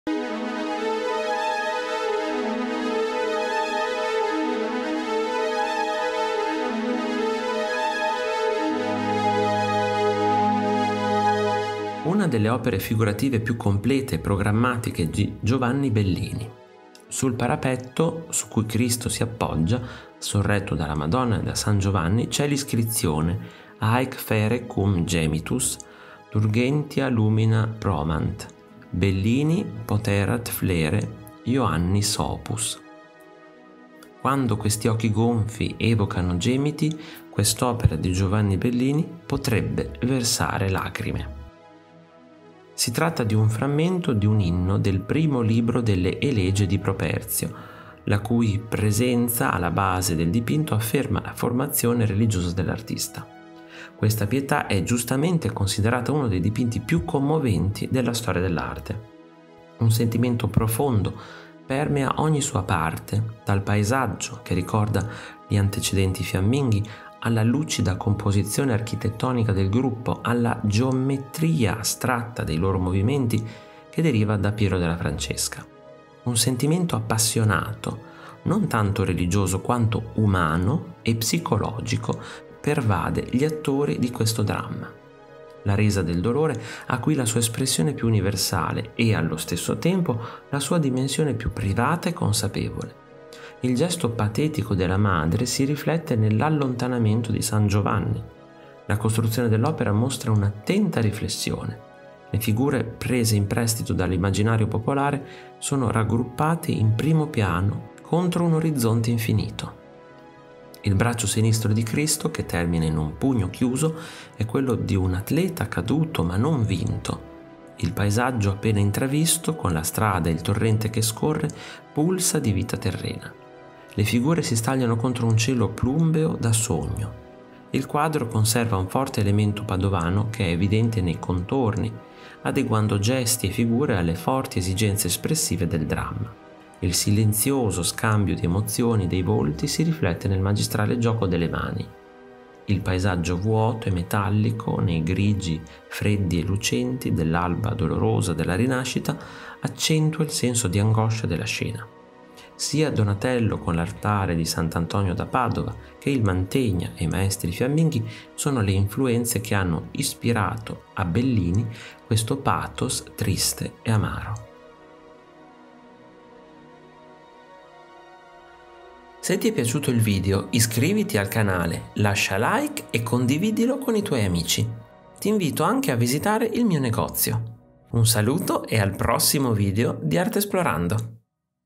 Giovanni Bellini | pietà
L'audioquadro è un modo per conoscere i più grandi capolavori della storia dell'arte. In maniera semplice e in pochi minuti. Qui vi parlerò della Pietà di Giovanni Bellini▼▼CONTINUA SOTTO▼▼

Una delle opere figurative più complete e programmatiche di Giovanni Bellini. Sul parapetto su cui Cristo si appoggia, sorretto dalla Madonna e da San Giovanni c’è l’iscrizione: HAEC FERE QUUM GEMITUS TURGENTIA LUMINA PROMANT: BELLINI POTERAT FLERE IOANNIS OPUS (Quando questi occhi gonfi evocano gemiti, quest’opera di Giovanni Bellini potrebbe versare lacrime). Si tratta di un frammento di un inno del primo libro delle “Elegie” di Properzio, la cui presenza alla base del dipinto afferma la formazione religiosa dell’artista. Questa Pietà è giustamente considerata uno dei dipinti più commoventi della storia dell’arte.

LETTURE CONSIGLIATE
➡ Giovanni Bellini. Catalogo della mostra (Roma, 30 settembre 2008-11 gennaio 2009)
➡ Donne e Madonne. Le sacre maternità di Giovanni Bellini
➡ Giovanni Bellini. La nascita della pittura devozionale umanistica